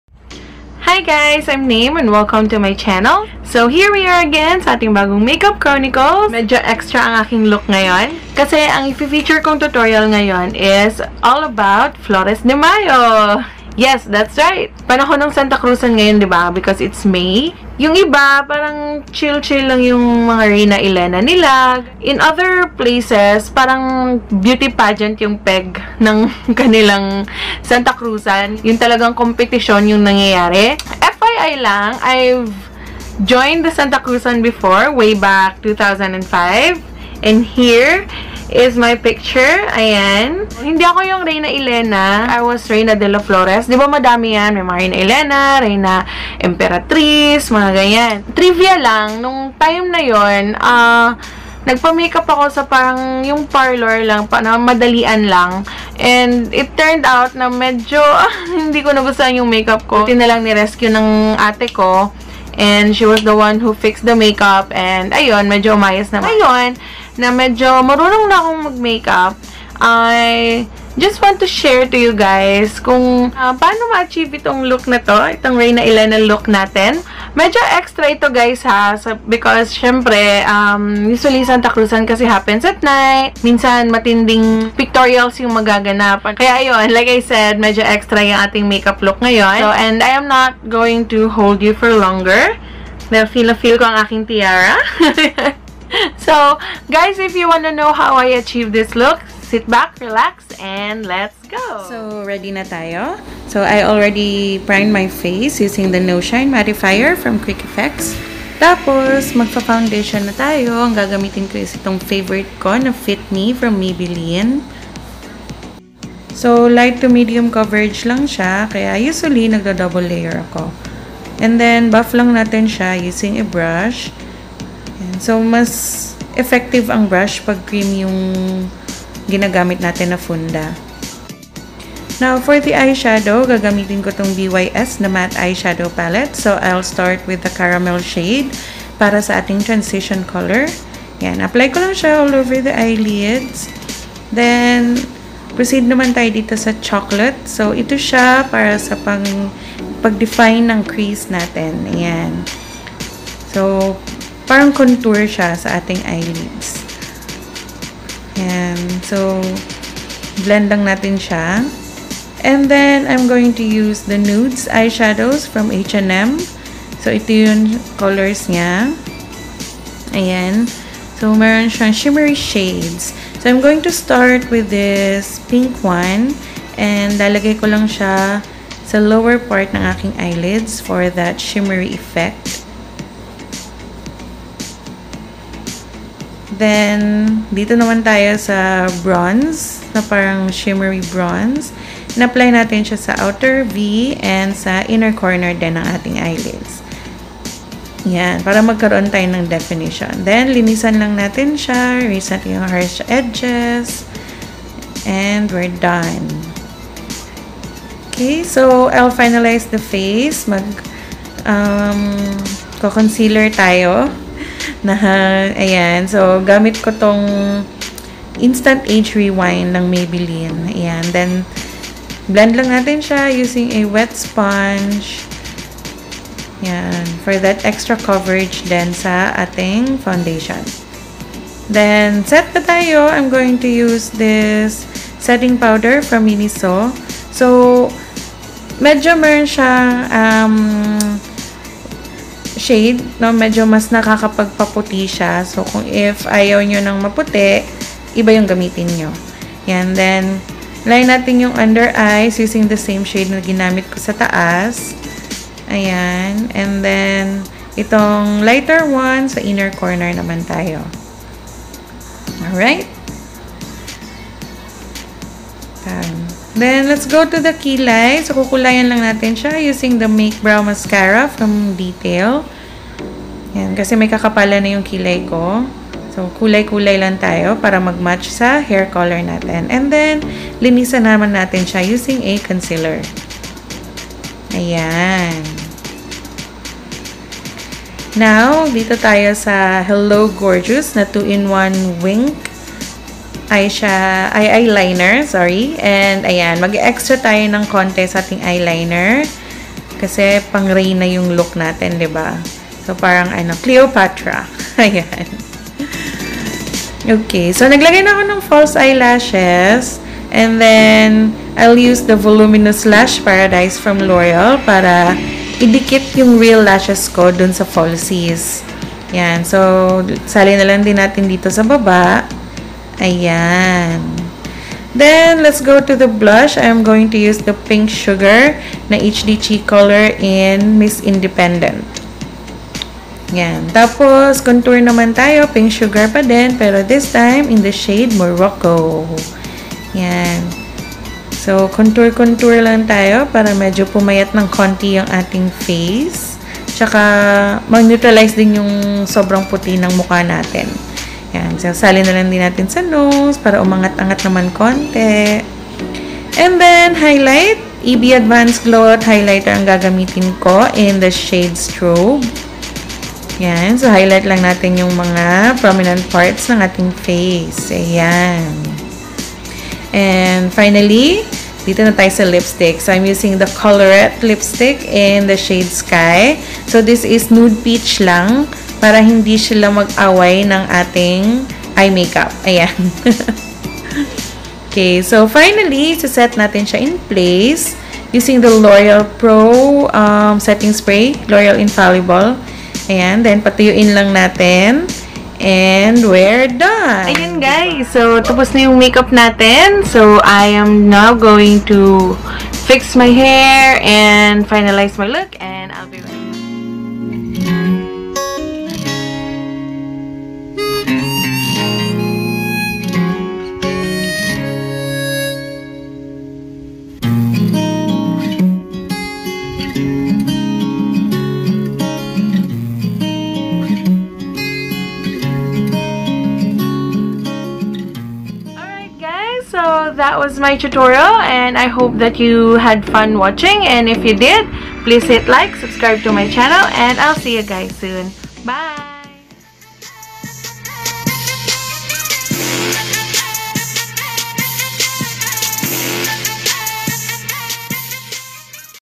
[MakeUp Chronicles] Flores de Mayo "Reyna Elena" Concept Tutorial
Ayan! Habol habol din sa patapos nang panahon ng Santacruzan! Hope you enjoy my quick tutorial. xoxo